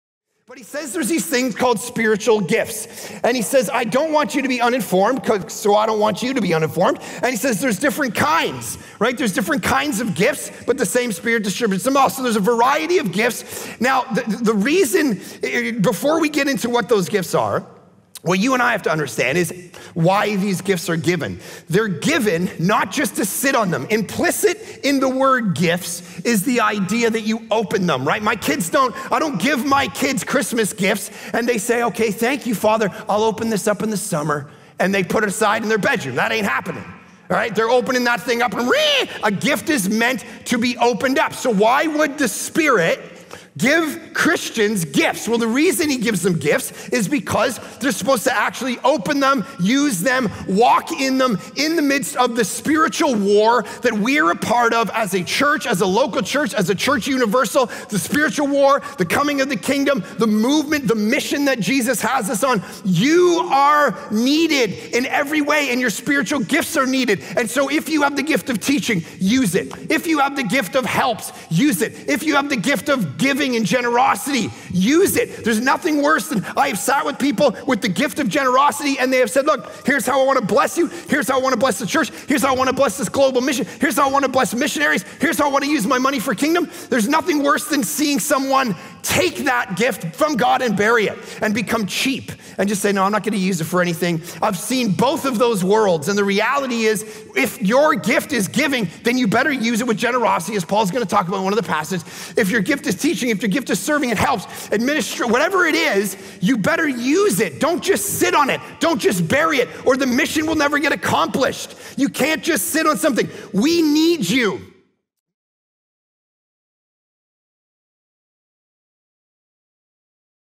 "You are needed in every way and your spiritual gifts are needed."
Sermon: Masterclass on Life 41: Spiritual Gifts (1 Corinthians 12:1-11)
Speaker: Mark Clark